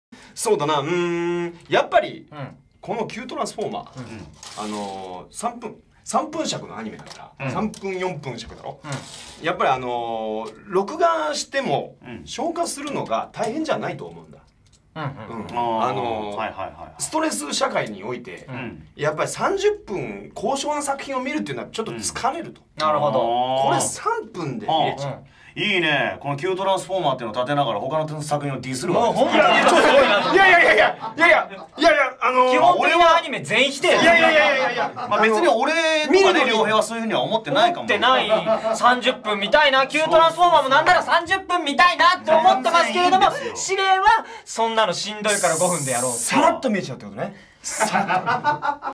[ENG SUBS] Hosoya Yoshimasa and Stress
Wanted to upload something so decided to do something short, I'm currently in the midst of translating something else though.

Featured Seiyuus
Kimura Ryouhei
Hosoya Yoshimasa
Suzuki Tatsuhisa

Date Aired
2015/07/03

Translation & Subtitles
Sonna Ii Kanji Subs

Korean Translation Source
일개노른자

Raw Video Source
DLE Channel